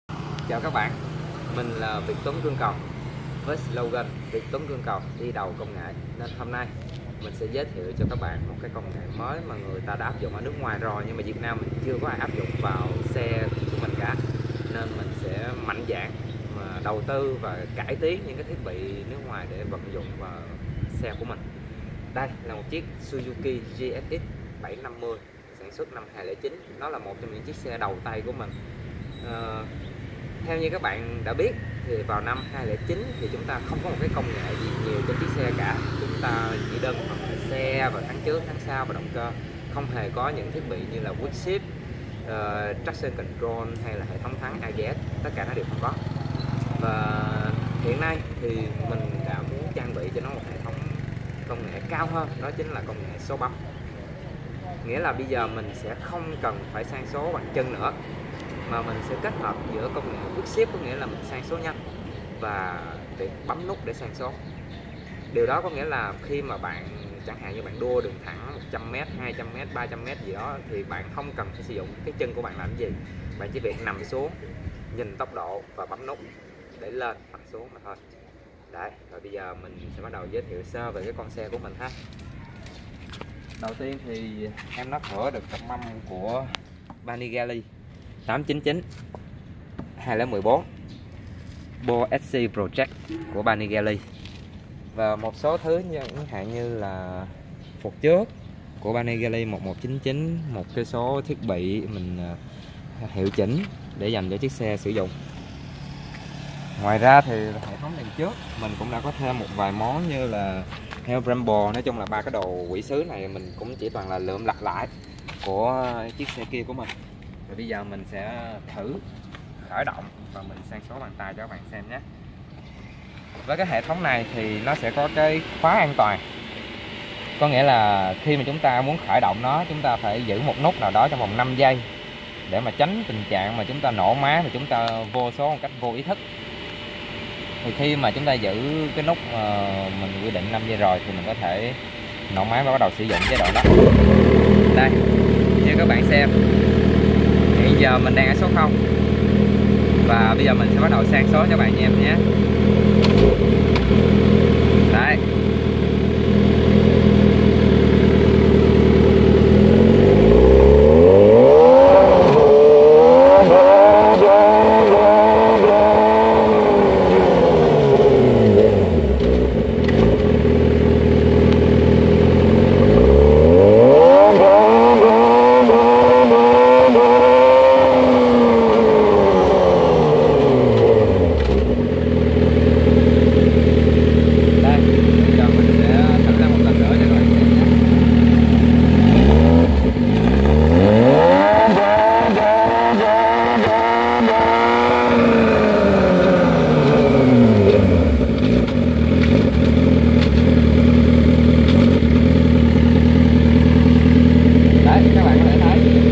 Suzuki Gsxr 750 Độ Sang Số Tay Bằng Nút Bấm - VietTuanGC Kĩ Sư Hẻm VietTuanGC
Kĩ sư hẻm VietTuanGC độ sang số tay vào chiếc Suzuki GSXR 750 2009 đầu tiên tại Việt Nam.
Áp dụng các linh kiện sẵn có trong và ngoài nước,VietTuanGC đã hoàn thiện bộ sang số bằng nút bấm đầu tiên tại Việt Nam.
Lên hoặc xuống số chỉ bằng nút bấm điện.
Không cần bóp côn cả khi lên và xuống số.
Tích hợp lên và xuống số nhanh.
Tự động vù ga khi xuống số.
Có thể sử dụng song song số móc truyền thống hoặc số bấm.